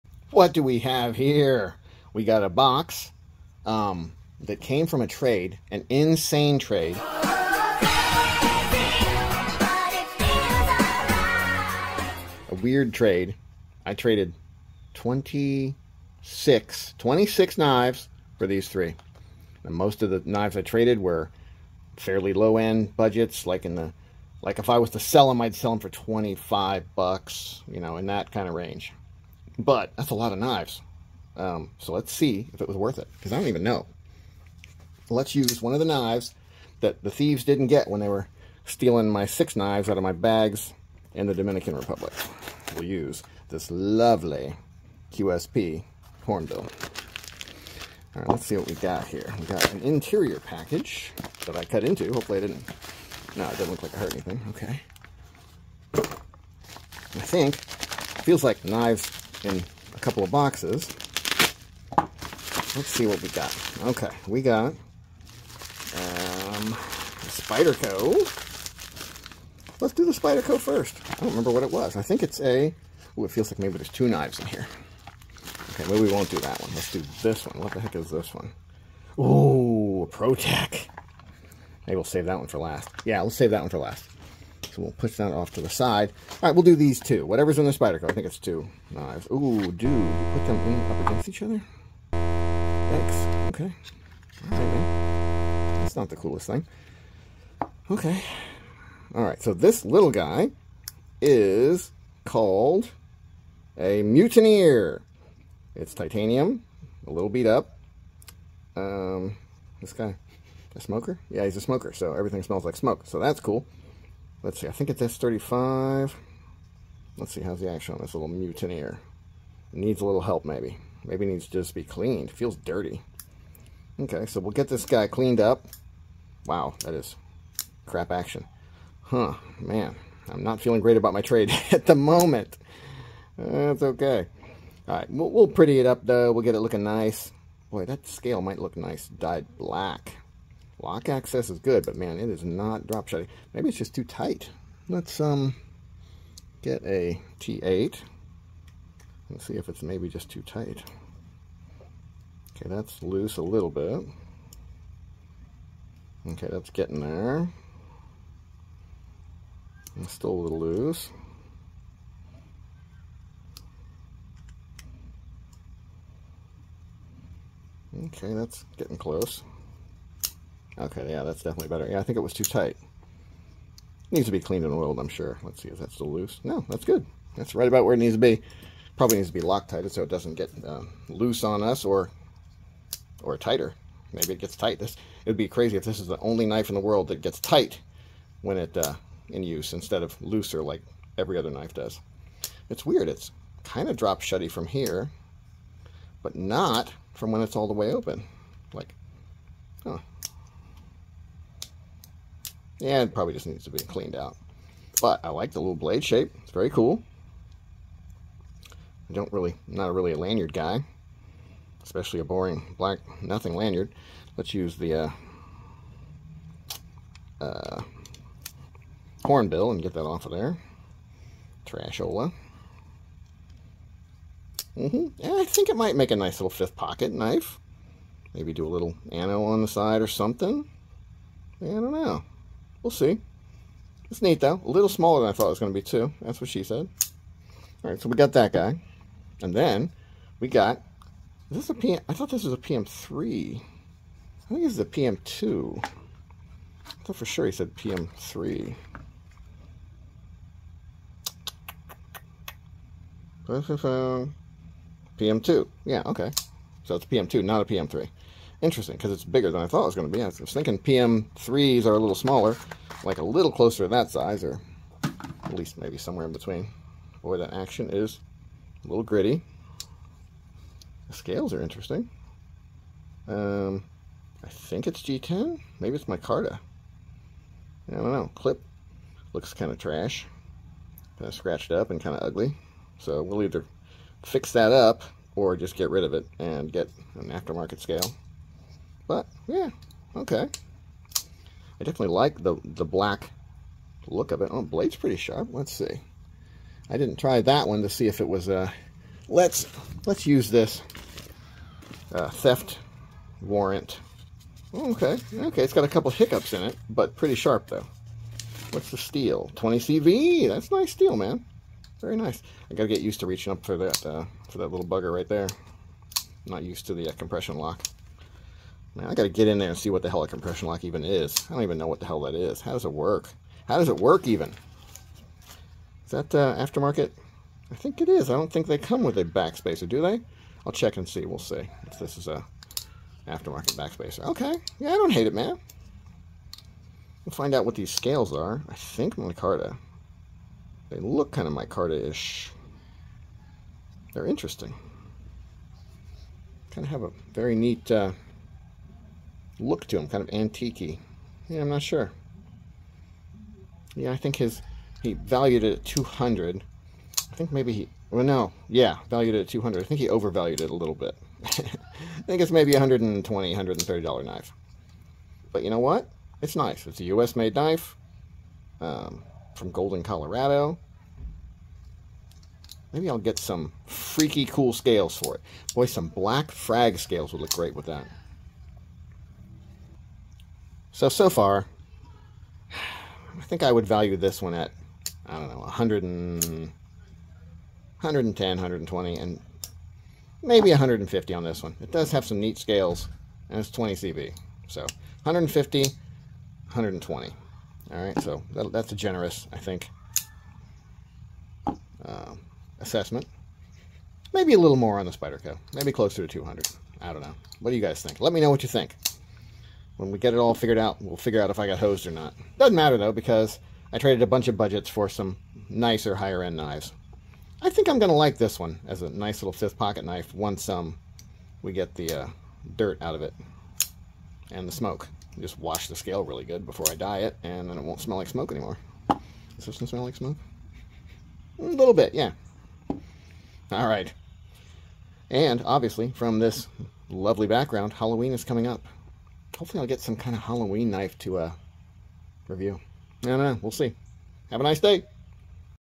Trading 26 knives for 3! Insanity or Brilliance? Unboxing Spyderco Picaroon Ti/Micarta Rate my Trade
You all know I have tons of budget knives, it's basically my thing. But I was going to sell a bunch I rarely mess with... Then a guy offered a trade, and I couldn't resist!

So let me know, rate my trade, etc. This is part 1, the Protech will be in part 2. So have a nice day!